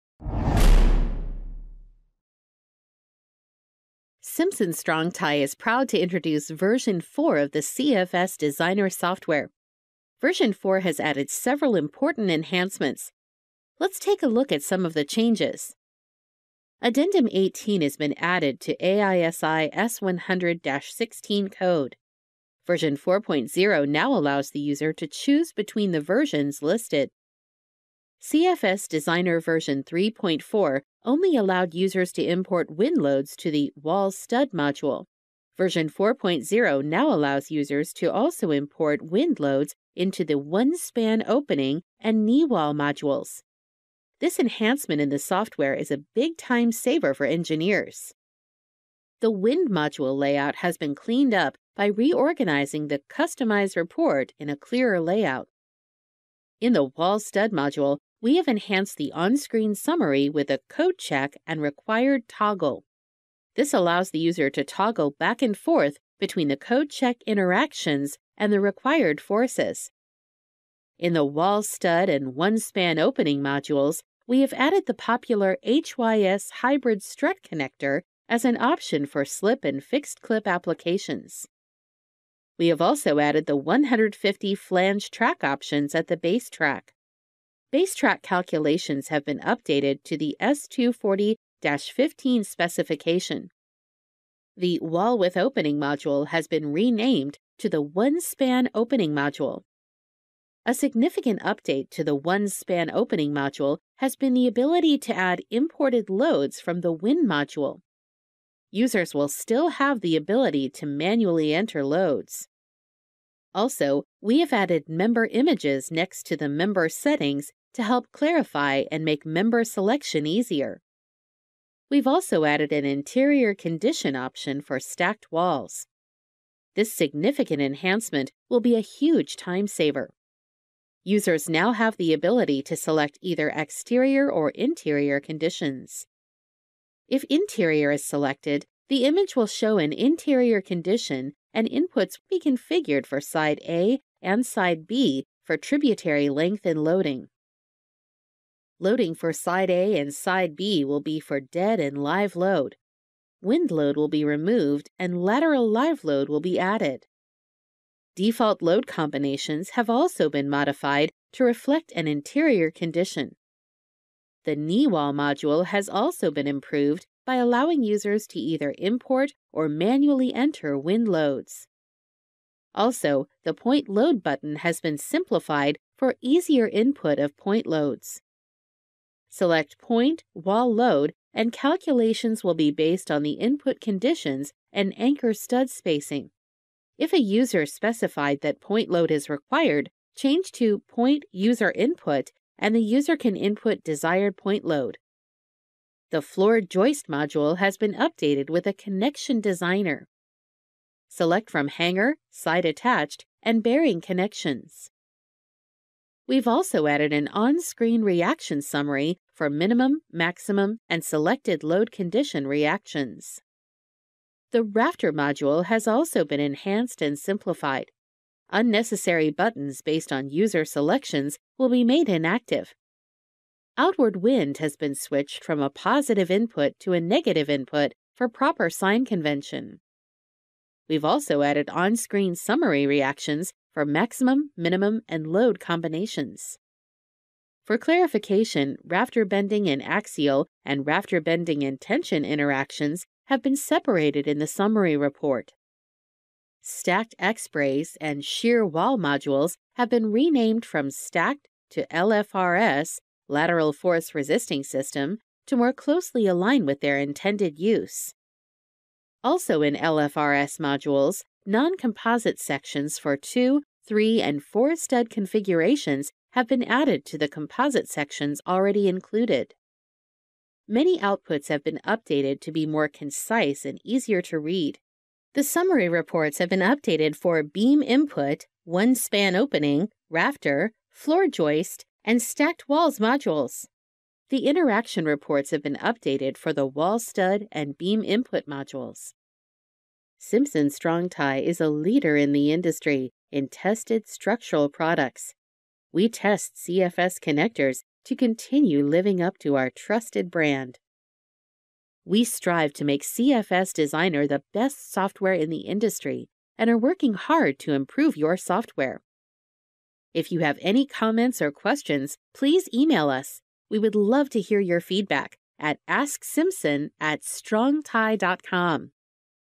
What's New with CFS Designer™ Software version 4 0
Simpson Strong-Tie® CFS Designer™ gives cold-formed steel (CFS) Designers the ability to design CFS beam-column members according to AISI specifications and to analyze complex beam loading and span conditions. Intuitive design tools automate common CFS systems such as wall openings, up to eight stories of load-bearing studs, compression posts for shearwalls, and floor joists with unbalanced live-load combinations. CFS Designer™ allows the design of multiple systems within the same job file, and supports connection design for specific Simpson Strong-Tie® curtain wall and bridging connectors like the SUBH u-channel bridging connector.

00:00 - Intro to CFS Designer Version 4.0
00:16 - Code: Addition of S100-16 w/ S1-18 amendments
00:27 - Import of Wind Load to 1-span opening and kneewall modules
00:48 - Cleaned up Wind Load Module
00:56 - Updates to Wall Stud Module
01:34 - Updates 1-Span Opening Module
03:15 - Updates to Floor Joist Module
03:35 - Updates to Rafter Module
04:06 - Updates to LRFS X-Brace and LRFS Shear Wall Modules
04:30 - General Output Updates
04:50 - Outro

Complete list of updates:
- Addition of S100-16 w/ S1-18 amendments
- Import of Wind load to 1-span opening and kneewall modules
- Enhanced wind module layout
- Additional on-screen report for required loads for wall stud module
- Added HYS and 150 track leg to connector selector
- Add images to member settings for 1-span opening
- Added interior/exterior condition option to stacked wall module.
- Enhanced point load selector in kneewall module
- Added connection selector in floor joist module
- Add a negative sign to outward load in rafter module
- Check Bending and Axial and Bending and Tension separately in rafter module
- Changed stacked shearwall and stacked x-brace modules to LRFS shearwall and LRFS x-brace
- Added non-composite 2,3 and 4 stud configurations to LRFS shearwall and x brace module.
- Update all outputs to be more concise, consistent and easier to read.